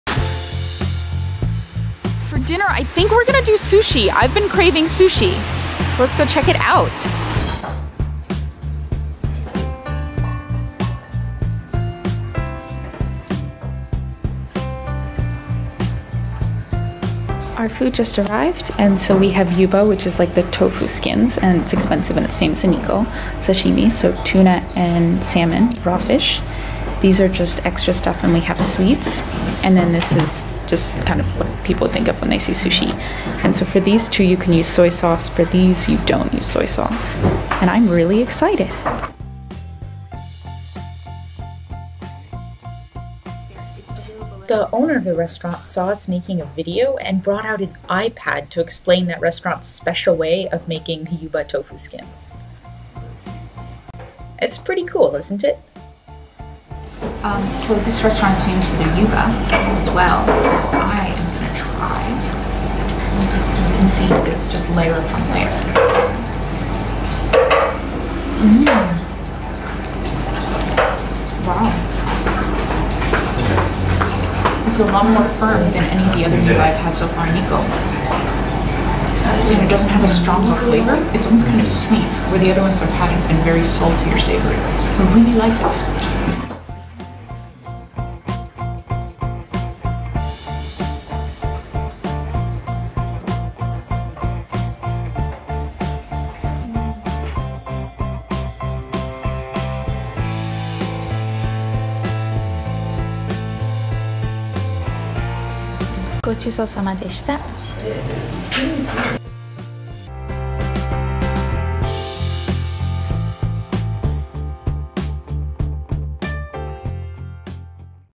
JAPANESE FOOD - Sushi and Tofu Skins
This is the last of my Nikko videos!
(Sorry I've been on such a food craze lately - food videos are totally the easiest to make, and y'all seem to really like them)